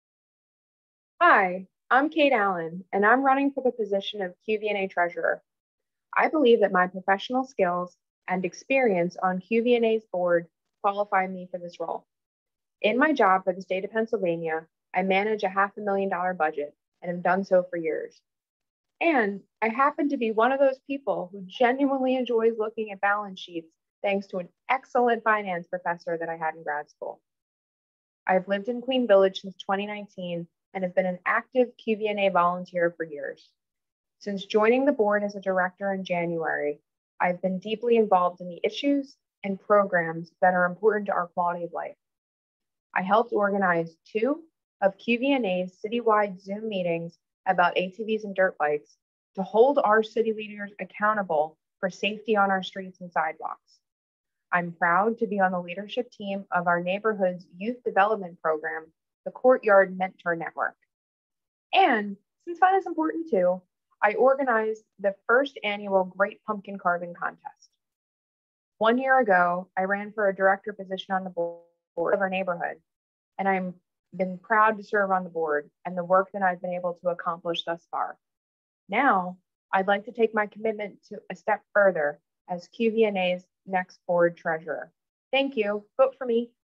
Cait Allen, TREASURER Candidate
I now serve as a Director on QVNA’s Board and would love the opportunity to step into the role of Treasurer. I work for the Patient Safety Authority, an independent state agency, as a department director and I personally manage a $500k budget. Prior to that, I worked with budgeting for two major Philadelphia health systems. My love of numbers began as an undergraduate economics student and continued through finance classes in grad school. I fell in love immediately with Queen Village since moving here three years ago and would love to further help our neighborhood.